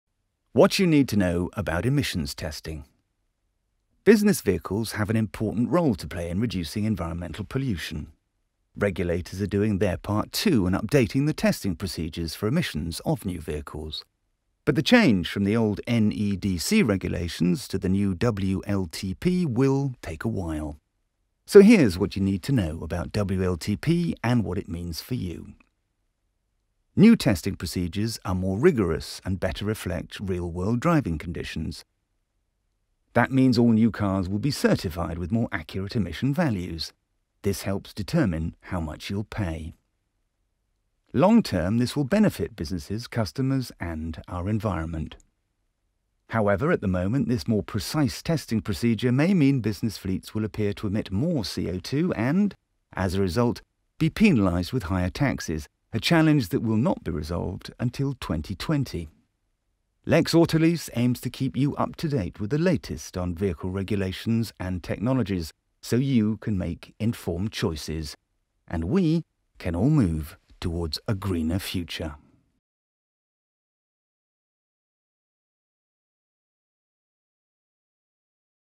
Vehicle Emissions testing: what you need to know
As the testing procedures for emissions of new vehicles are updated, here's our summary of the Worldwide Harmonised Light Vehicle Testing Procedure (WLTP) and what it could mean for you.